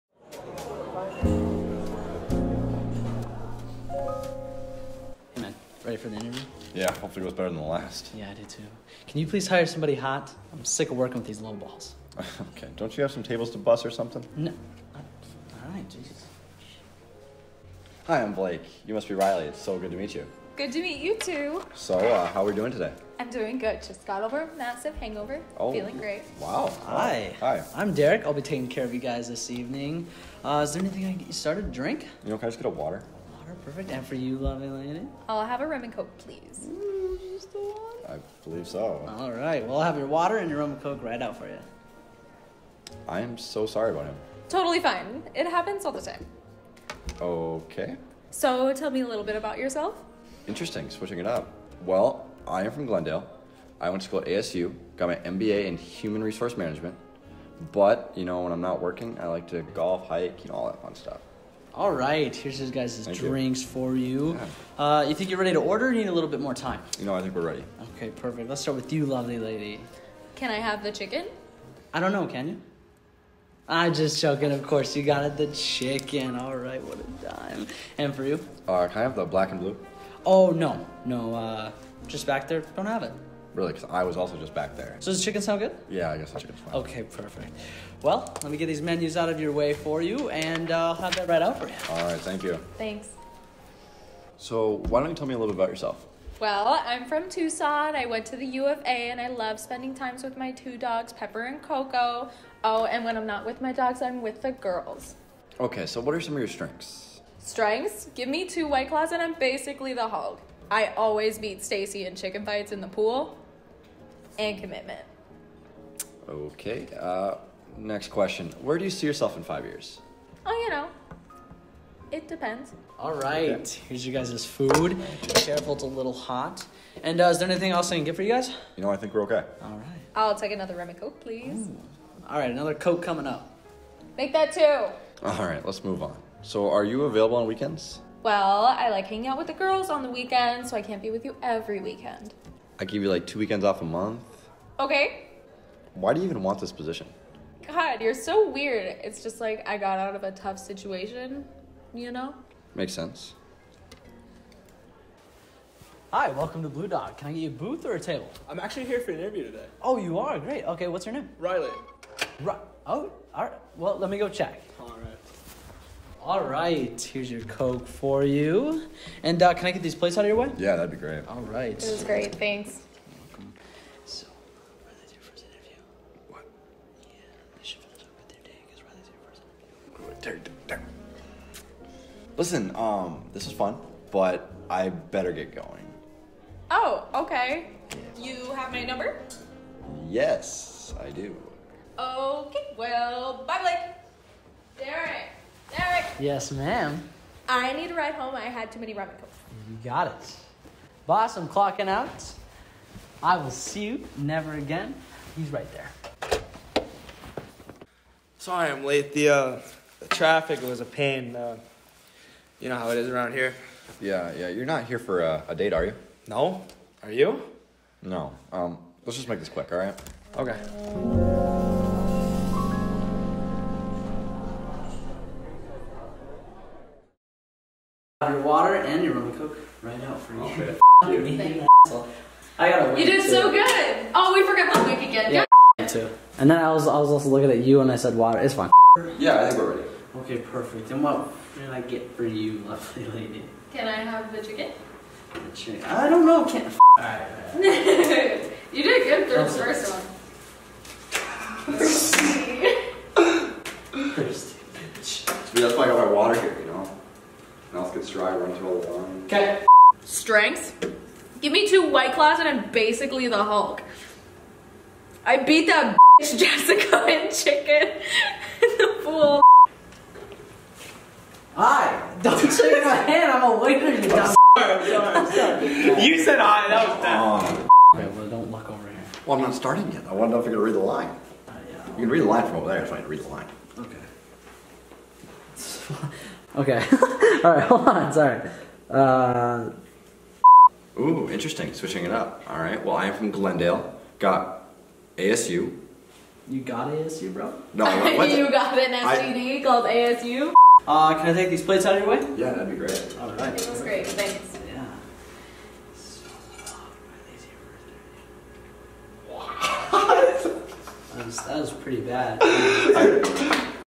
First Date.
...Rylie doesn't understand that this is an interview and Blake doesn't understand that she thinks its a date...

Description:
Rylie (Bailee R.) is a flirty women about to go on a tinder date when all of the sudden she crosses paths with a local restaurant owner. Blake (Brennen W.) is looking for an interviewee for a new position opening up here at the Tavern. When these two cross paths, I think they both learn something new. Rylie isn't the shy type or nervous for this interview at all. Rylie doesn't understand that this is an interview and Blake doesn't understand that she thinks its a date. Derick (Cade R.) is a waiter and loves just hanging out. He wants Blake to "hire someone hot". He has his own motivations, and eventually gets confused if its a date or an interview but he keeps going!  then there is the real Rylee (Miller). He is the actually interviewee and gets treated poorly when Blake is sick of what just happened. We hope you enjoy!

CAST:
Rylie Bailee ReAnn
Blake Brennen Weiner
Derrick Cade R.
Rylee Matt Miller